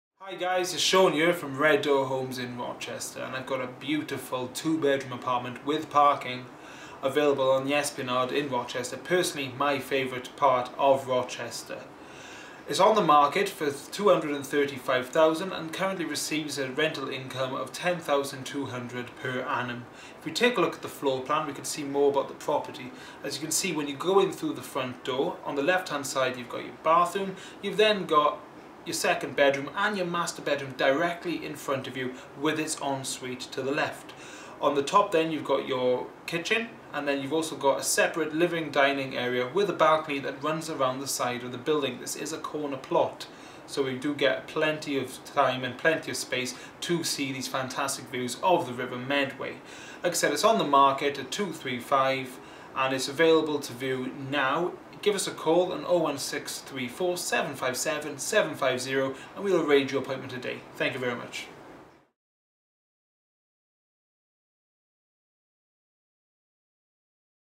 Keating Close, Rochester
Sean from RedDoor Homes presents you with a stunning 2 bed apartment in Rochester. Available from April 2016.

** CLOSE TO TRAIN STATION  ** FLOOR PLAN ATTACHED ** VIDEO TOUR AVAILABLE ** 2 Bedroom apartment with amazing views. This modern apartment has a spacious lounge/diner with French doors leading to a balcony to the front of the apartment over looking the river front, along with another set of French doors also sharing the same views. From the master bedroom again there is a balcony overlooking the green area to the side of the property. Along with its fantastic views the property has an en-suite, modern kitchen, family bathroom, storage and also allocated parking. The property is currently tenant and is being offered for sale with tenants in situ.